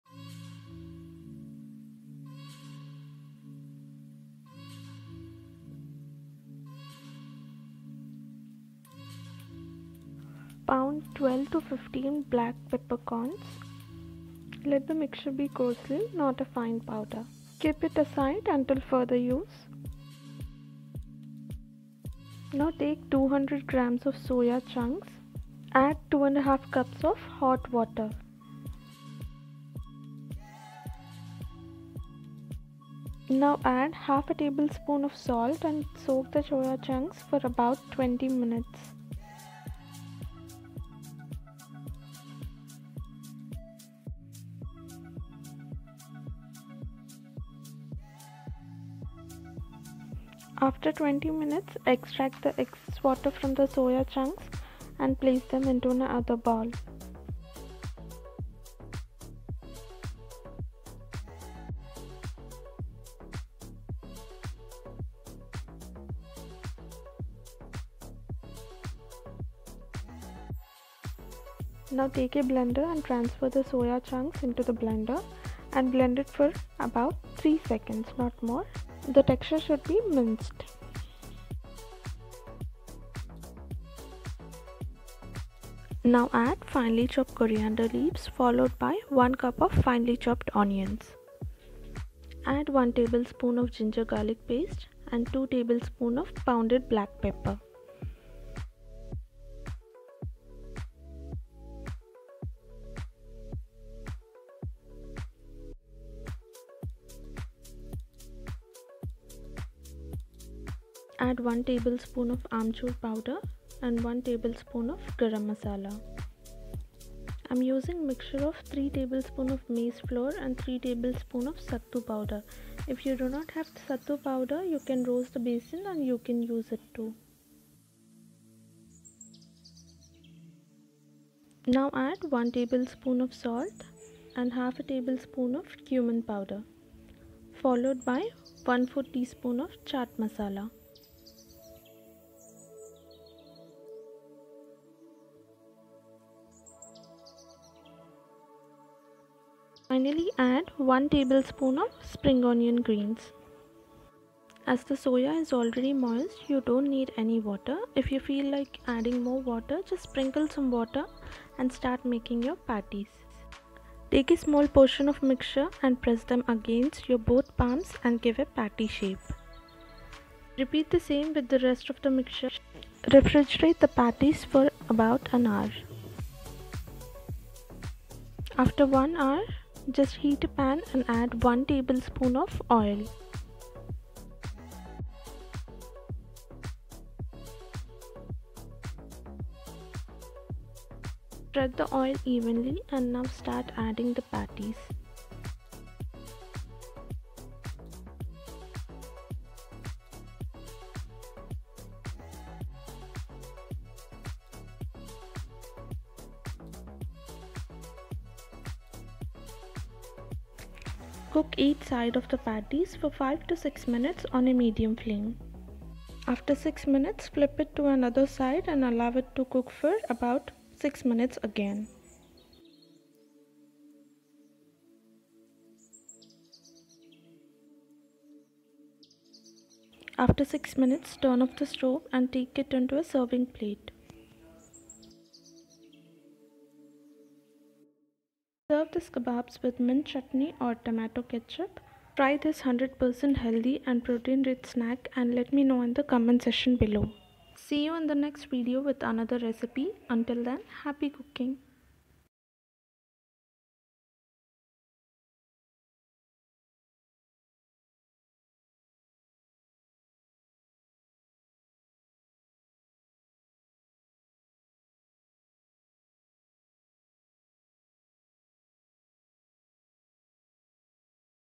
Soya Chunks Pepper Kababs
This protine rich snack just takes 0.5 hours to prepare. Maze flour is rich in fiber and Sattu is a great substitute for protein. Do try this amazing healthy recipe and mention in the comments below.

For more healthy and tasty recipes follow Zesty Cooking#rajsri. ❤️❤️❤️❤️❤️❤️❤️

Musician: Jef